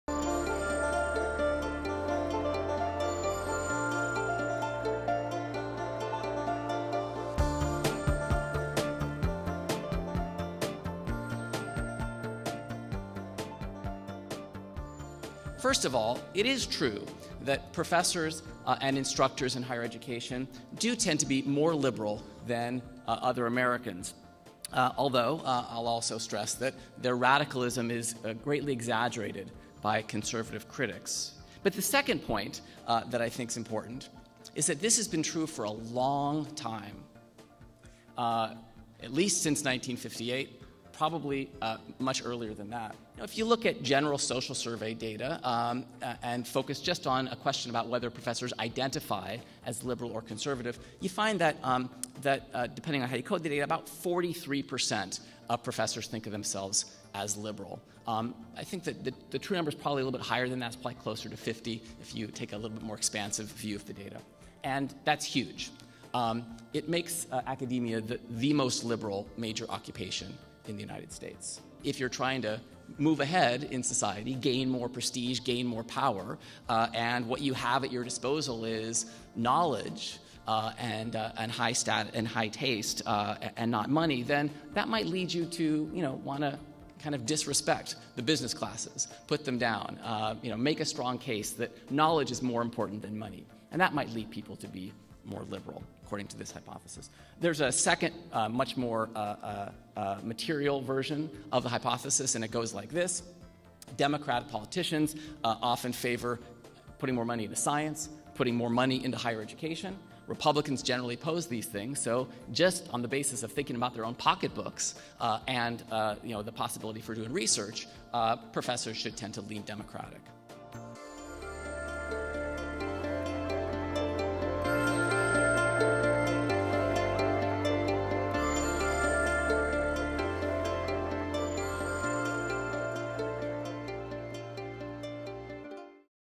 Common Hour: Why Are (Your) Professors Liberal? (Highlights)
Neil Gross, professor of sociology, University of British Columbia, offers his evidence and analysis while also engaging with F&M faculty and students in a dialogue on the interplay of ideology, politics, and polemics in the teaching and learning that takes place on a liberal arts campus such as F&M. Recorded Jan. 16, 2014, in Mayser Gymnasium.